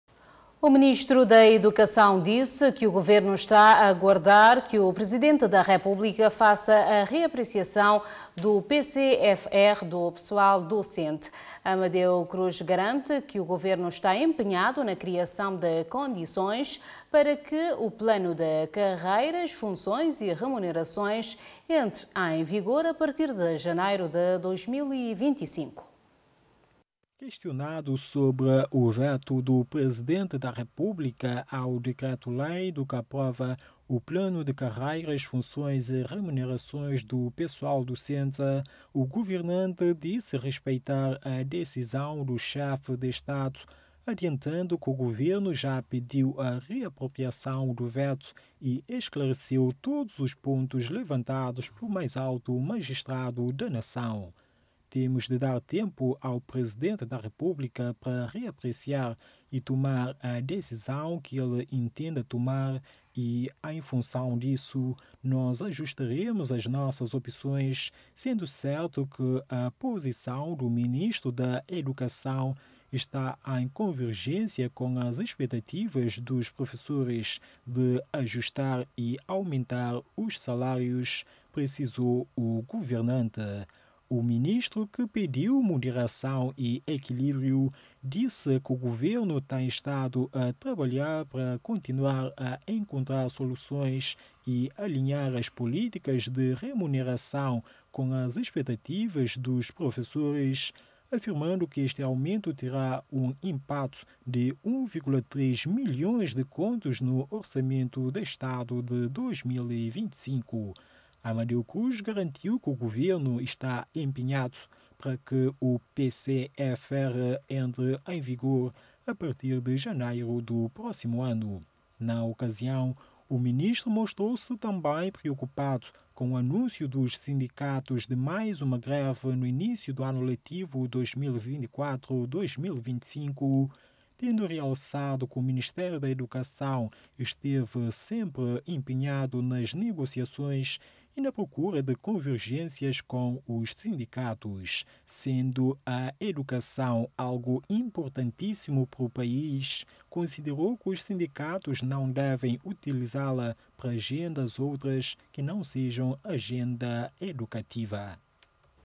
Governo aguarda nova análise do Presidente da República sobre PCFR da classe docente | Fala CV
Governo aguarda pela nova análise do Presidente da República ao Plano de Carreiras, Funções e Remunerações da classe docente.